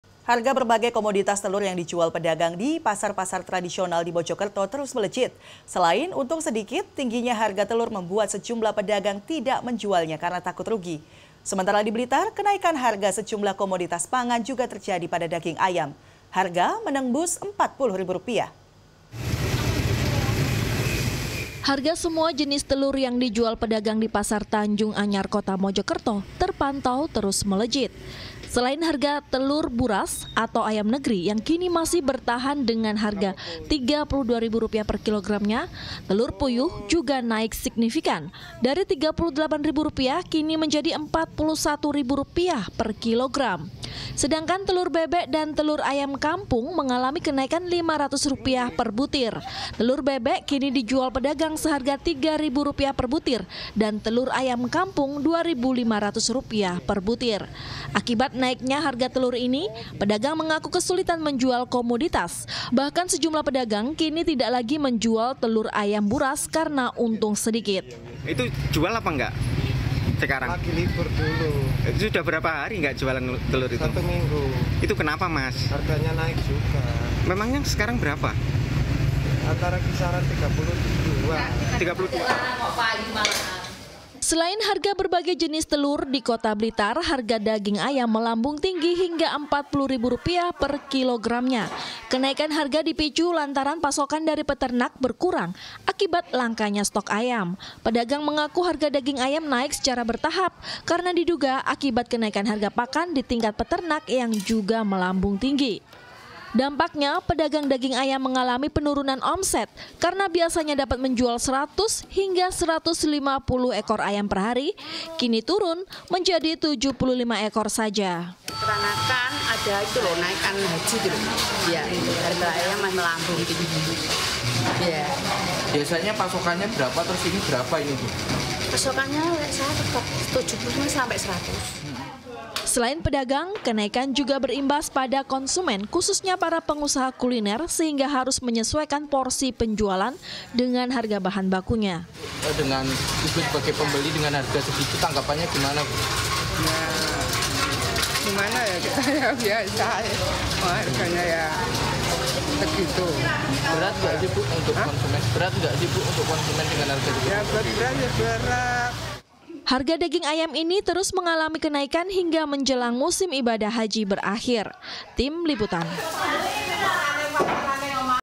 Harga Komoditas Telur Dan Daging Ayam Melejit
Harga  berbagai komoditas telur yang dijual pedagang di pasar-pasar tradisional, di Mojokerto terus melejit. Selain untung sedikit, tingginya harga telur membuat sejumlah pedagang tidak menjualnya karena takut rugi. Sementara di Blitar kenaikan harga sejumlah komoditas pangan juga terjadi pada daging ayam, hingga menembus 40 ribu rupiah.